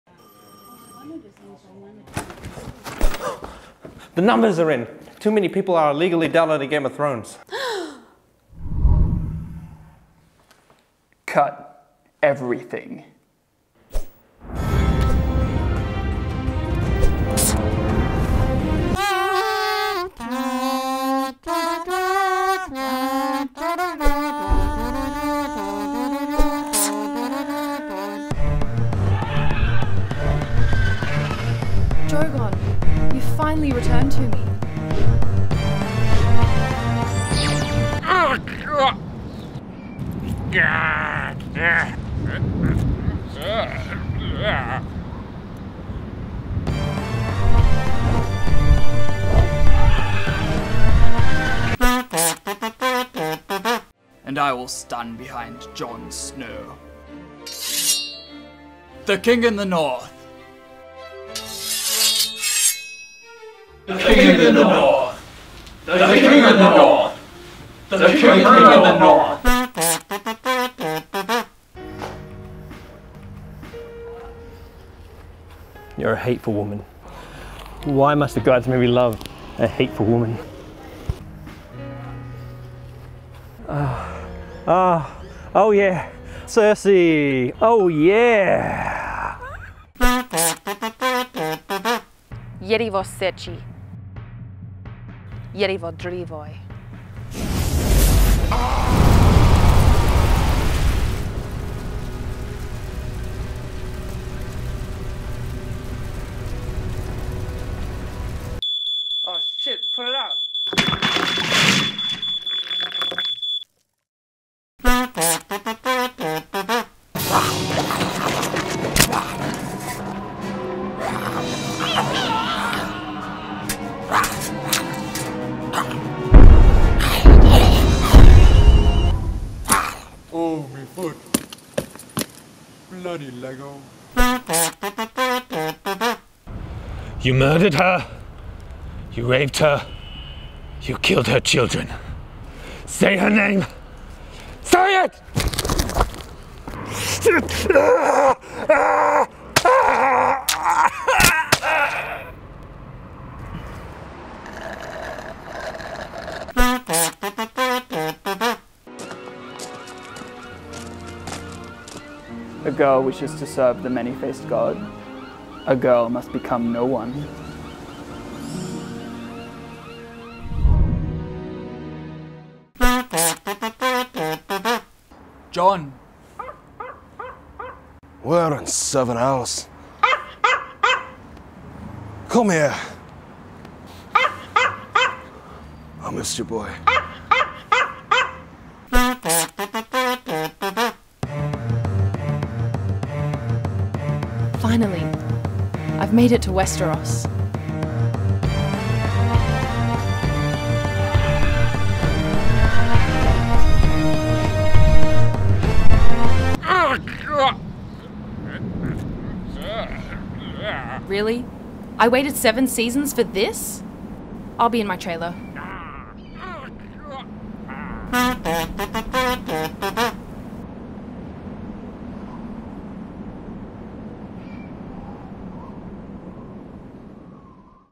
Game of Budget Cuts [Game of Thrones Parody]
Last year a local distributor held a film competition about the effect that piracy has on the film & television industry (fun fact: there are many reports that it actually helps boost ticket sales because it creates word of mouth, but of course the distributor didn't want to hear that). For our entry, we decided to reimagine one of the most pirated TV shows as a low budget series. We didn't win the competition. But now that Season 7 is about to be released we thought we'd share our video anyway!

► Drogon: Zeek
► Jaime: Sparta Cosplay

Written by Elliot Ryan
Directed by Elliot Ryan
Cinematography by Goldie Soetianto
Editing & VFX by Elliot Ryan
Colour Grade by Goldie Soetianto
3D Models Rigged by Reuben Jurott
BTS: Andy Diep, Elliot Ryan

Follow Deerstalker Pictures!